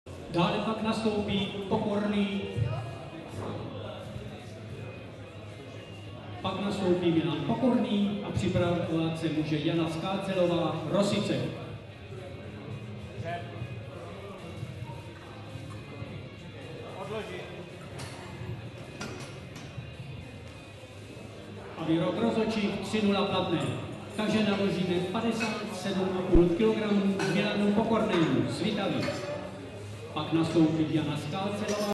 Lytvynyšynová Marija (2001), DŘ1, 55 kg
25. Mistrovství Jižní Moravy v silovém trojboji mužů a žen

Místo: Hornický dům, Zbýšov